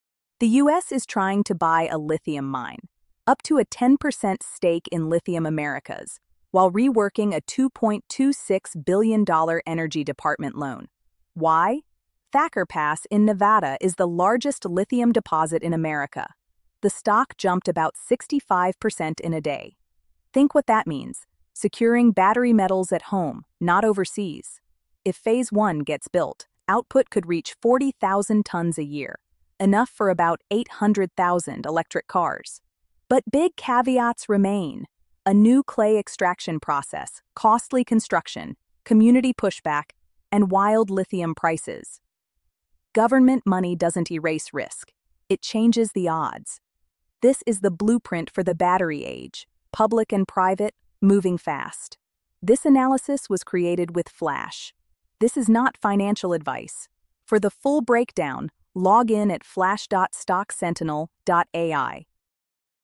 The U.S. Might Buy a Lithium Mine: Why LAC Spiked
The U.S. may buy into a lithium mine—here’s what it really means.

Reports point to a potential 10% U.S. stake in Lithium Americas tied to a $2.26B DOE loan. But a lithium mine still faces tech, permit, and price risks, so government money changes the odds, not the hurdles. We show how this lithium mine could supply ~800,000 EVs a year—fueling the domestic battery supply.

Analysis created with Flash using real filings, transcripts, and live financial data.